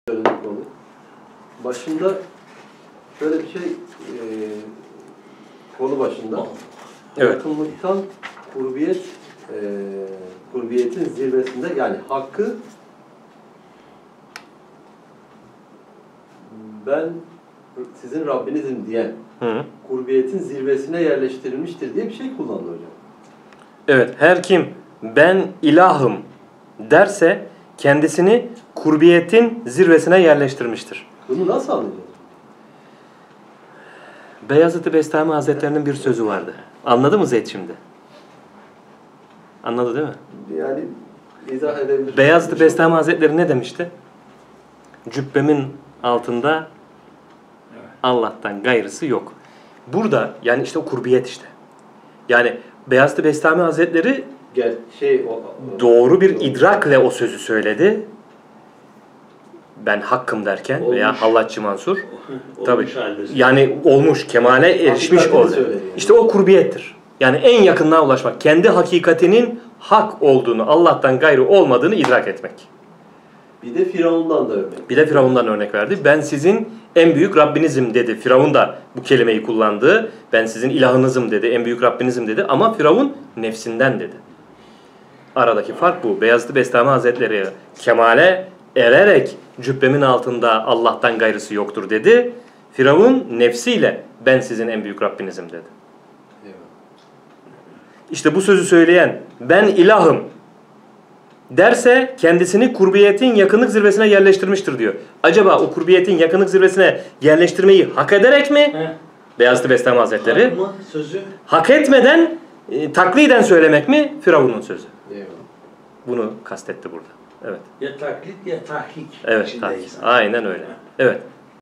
Firavun ile Beyazıt i Bestami Hazretleri arasındaki fark nedir?
Videonun Bulunduğu Aralık: Fütuhat-ı Mekkiyye 331. Bölüm Sorular ve Cevapları: 1:02:07-1:04:10

Not: Muhyiddin İbnü'l-Arabî Hazretleri (K.S)  Ekolü Üzere
Tasavvuf Sohbetidir.